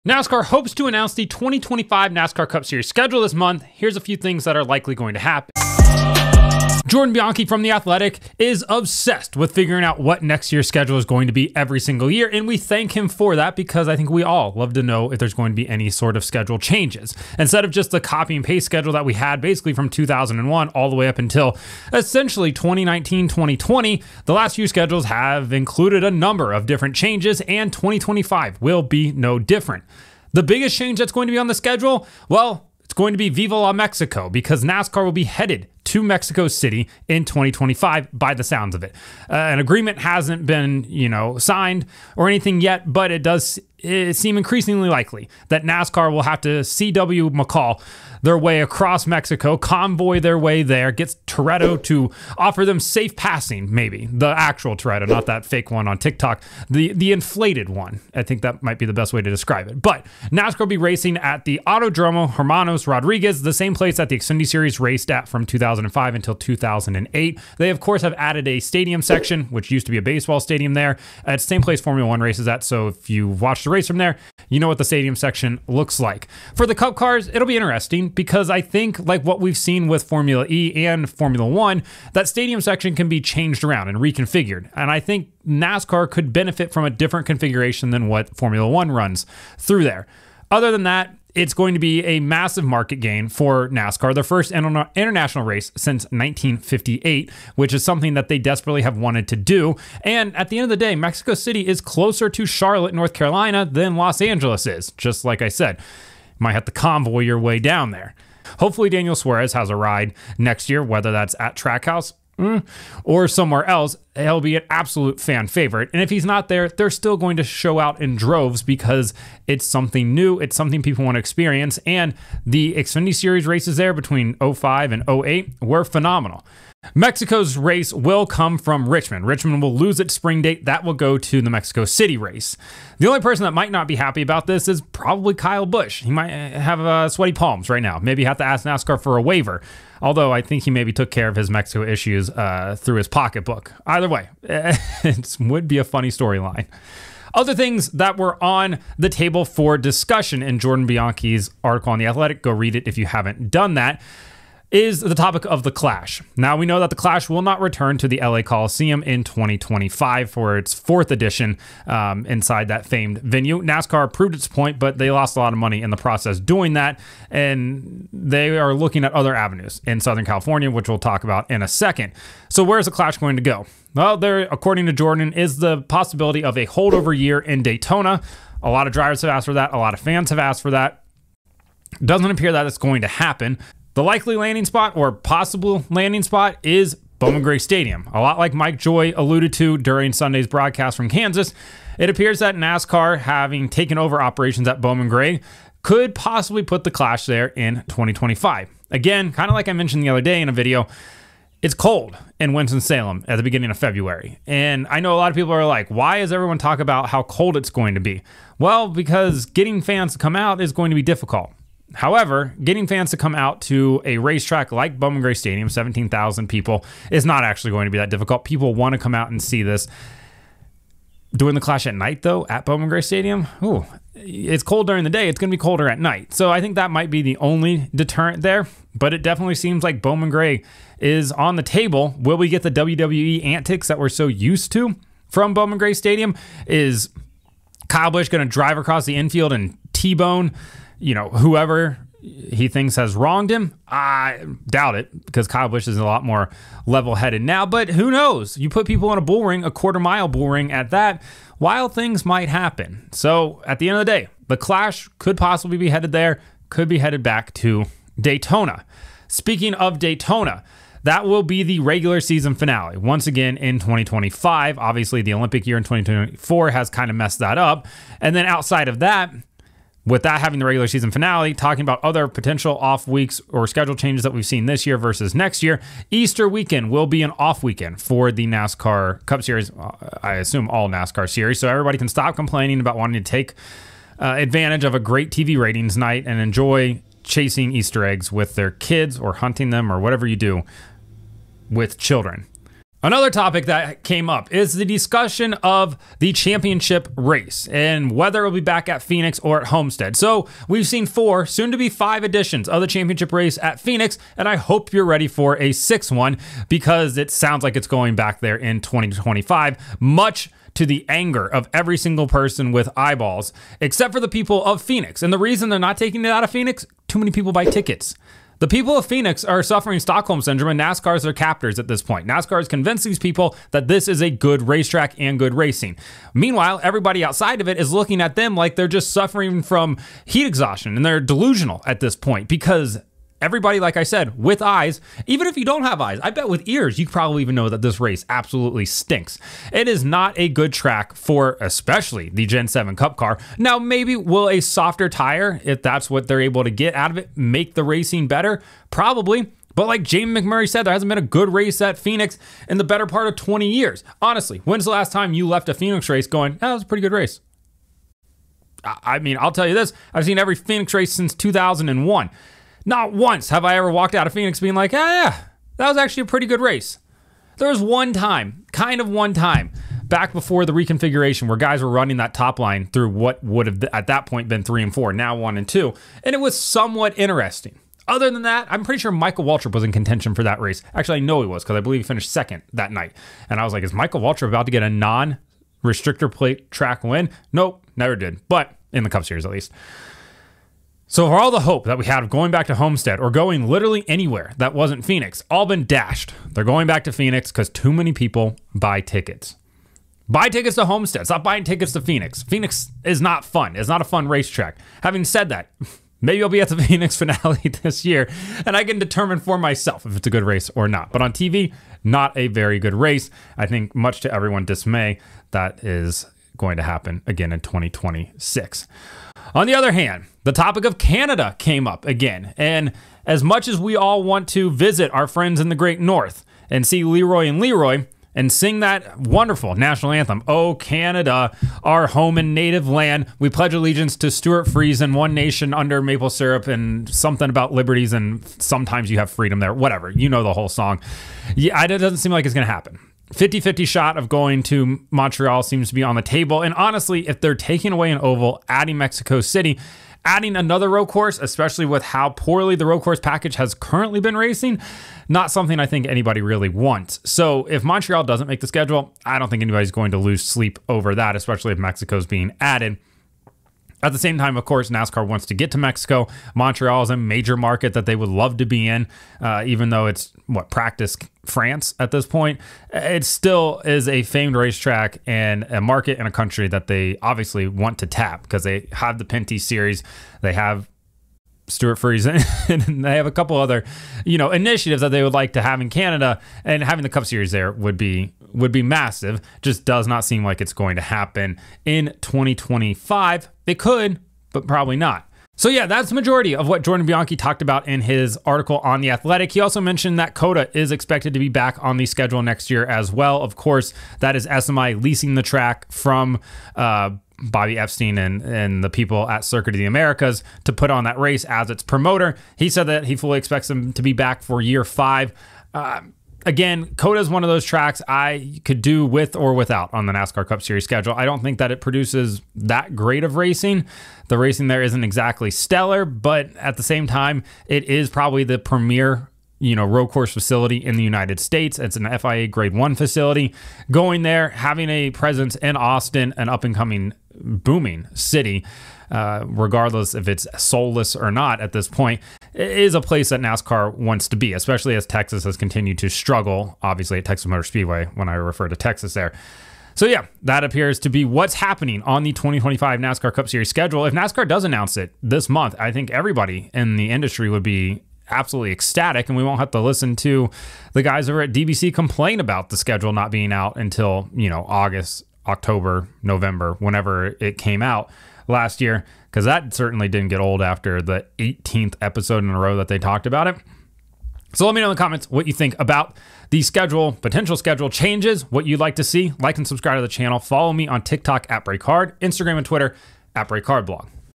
Breaking Down The 2025 NASCAR Cup Series Schedule Rumors
Breaking Down The 2025 NASCAR Cup Series Schedule Rumors. Is NASCAR finally headed to Mexico. What about Canada? Where will the clash be at in 2025 and will they finally have Easter off again?